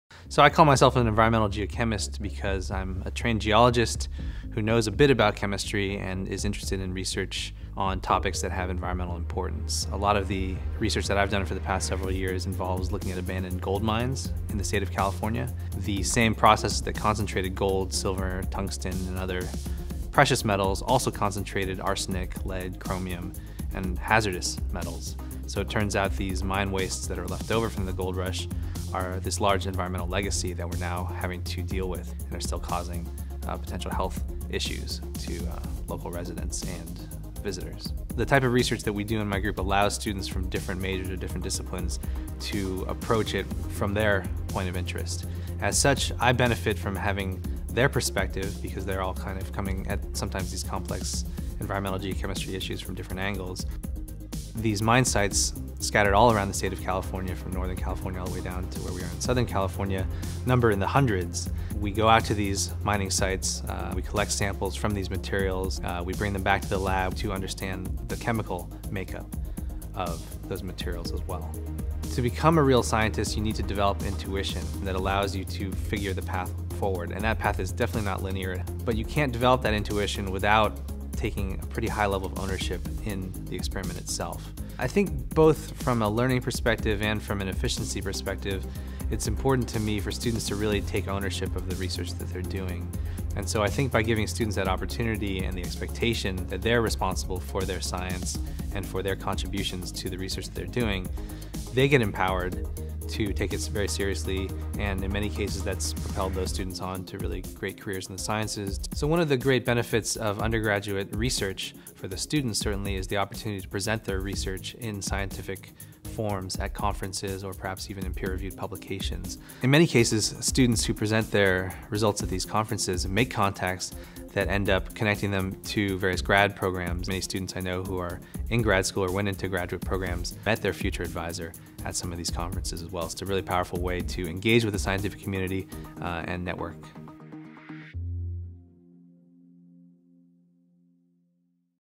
Dr. Christopher Kim – Environmental Geochemistry
The interdisciplinary bachelor of science in environmental science & policy creates scientists who can communicate with policy makers and future policy makers who understand science.